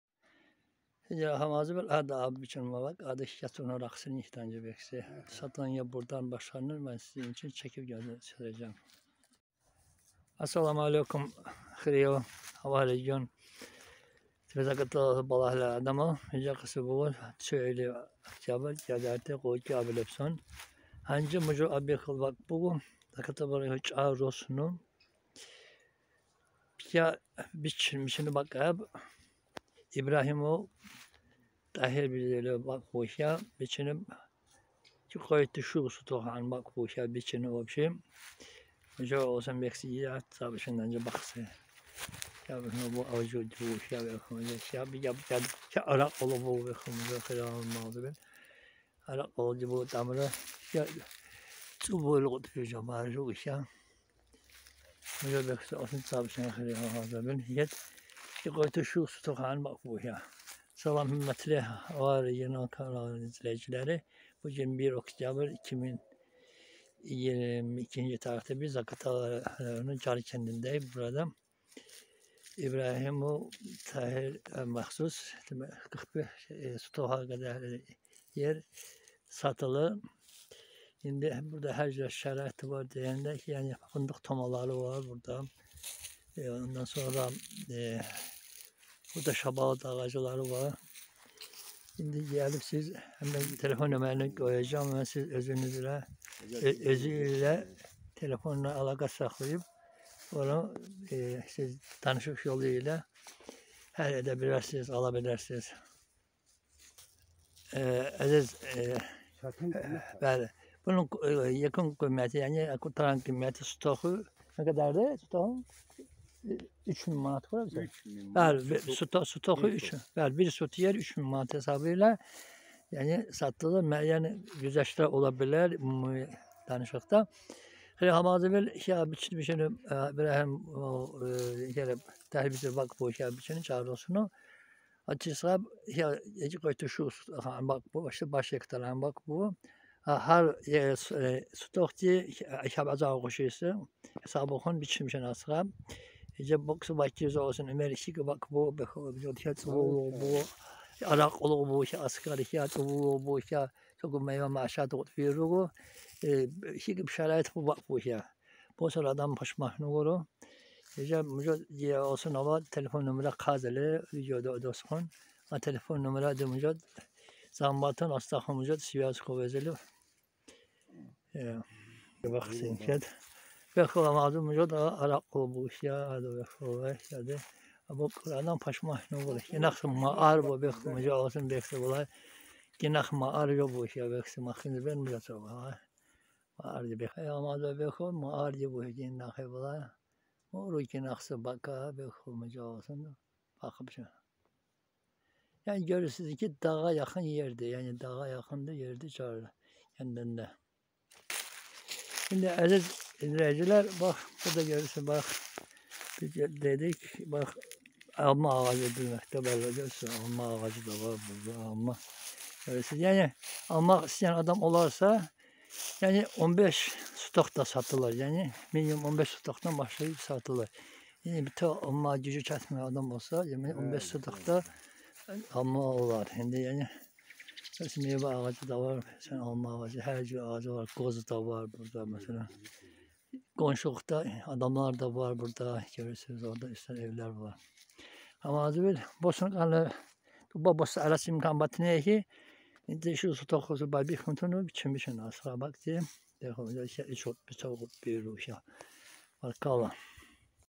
Срочно продаётся: земельный участок в селе Джар Закатальском Регионе _ 01-10-2022 года ❤️❤️❤️
Təcili satılır: Zaqatala Regionun Car kəndindəki Tahir İbrahimova məxsus 0,45 ha ( 45 sоток) torpaq sahəsi satılır 🌟🌟🌟Əlaqə nömrəsi_ Номер для связи: + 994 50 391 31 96 🤝🤝🤝 1 sotoku 3 000 az.m.💒💒💒Satış azı 15 sotokdan başlayır _ yəni, hissə-hissə də satılır 🌟🌟🌟